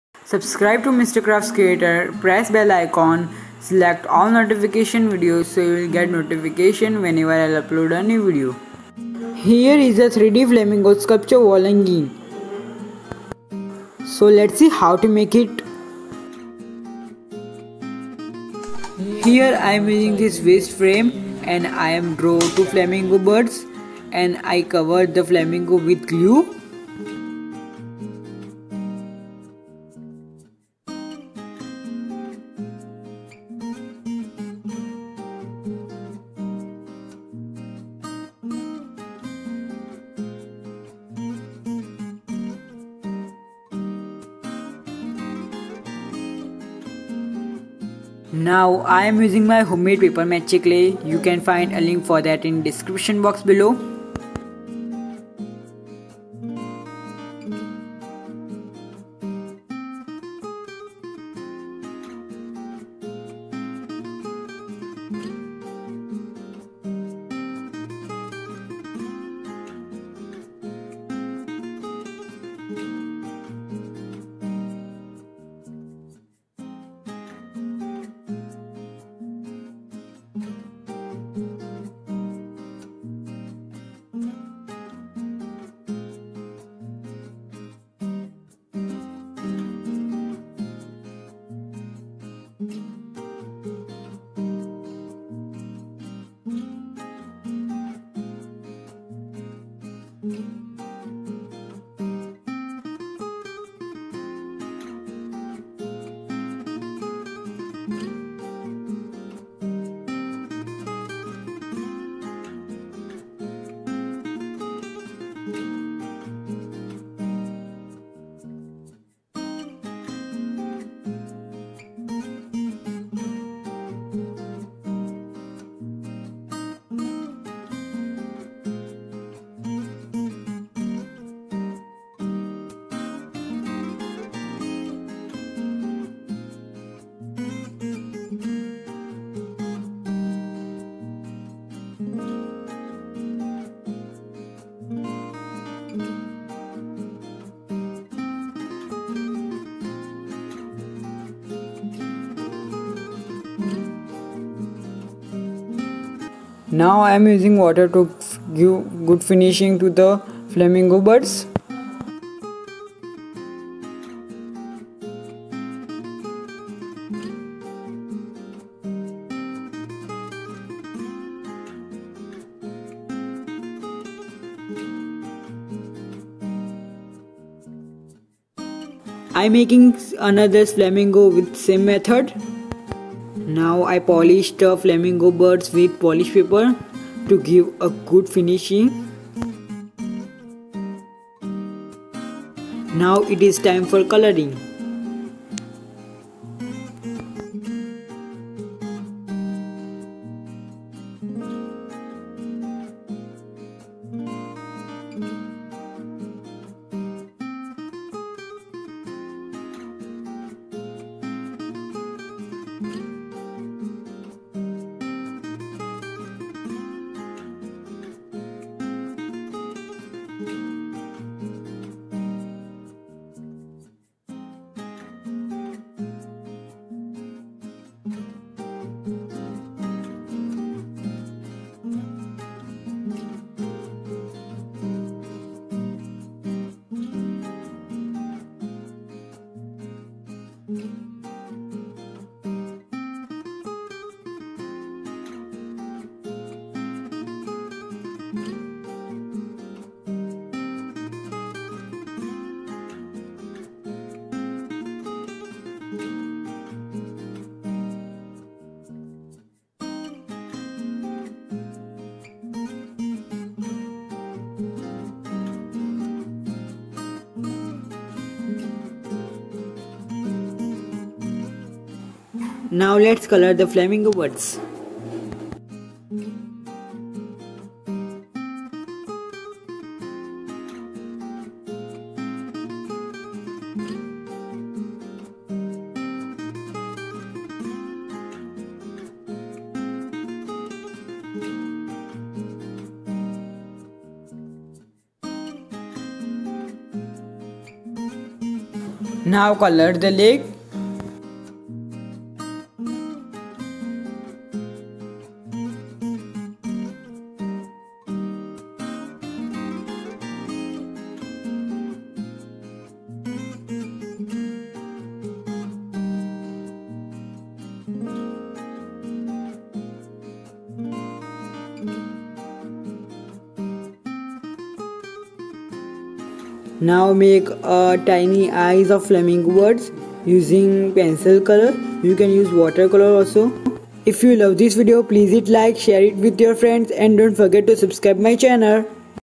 3D flamingo sculpture wall hanging/paper mache wall hanging/Mister crafts creater
Today in this tutorial I am going to show you how to make a 3D Flamingo sculpture wall hanging with paper mache clay  .
you can find link for Instagram I'd and enjoy Mister crafts creater channel

Acrylic colours